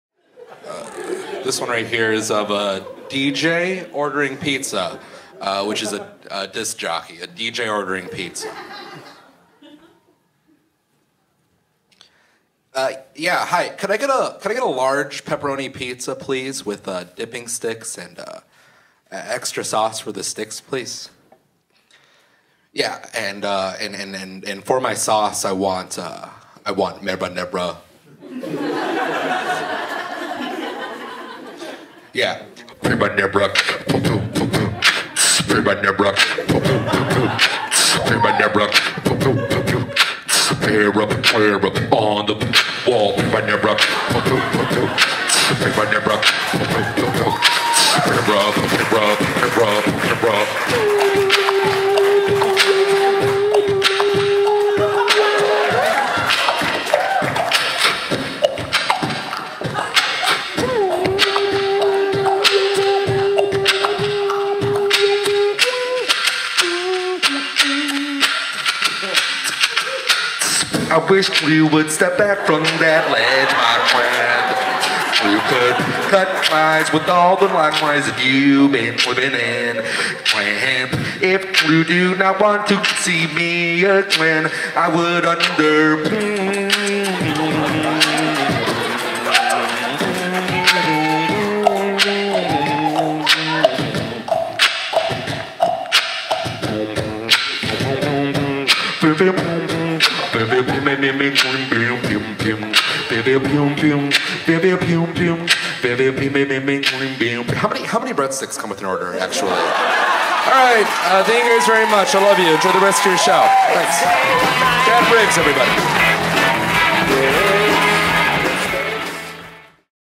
Sammy Arechar at Chicago Underground Comedy
Sammy Arechar drops some dope ass beats at Chicago Underground Comedy on November 4th, 2014.